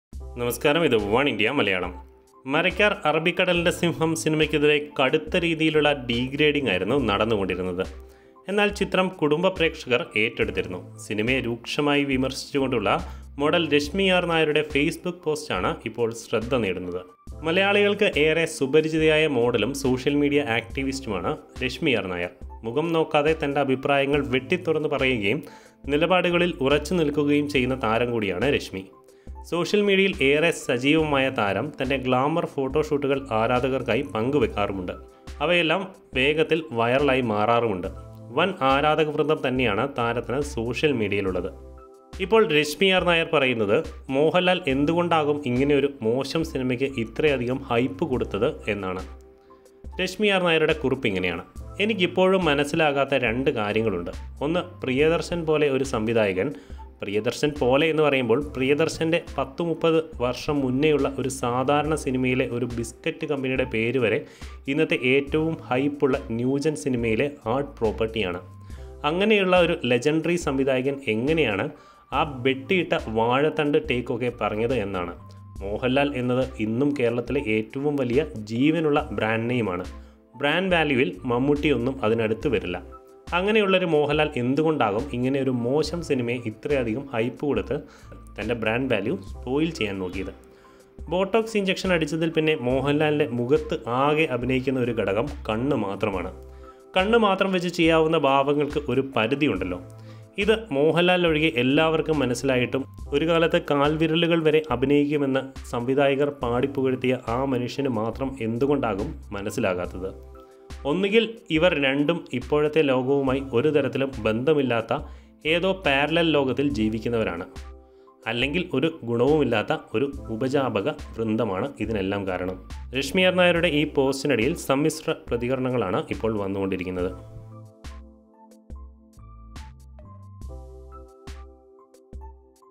ബോട്ടോക്സ് ഇൻജെക്ഷൻ അടിച്ചതിൽ പിന്നെ മോഹൻലാലിന്റെ അഭിനയം കണ്ണിൽ മാത്രം
Resmi Nair's criticism of Mohanlal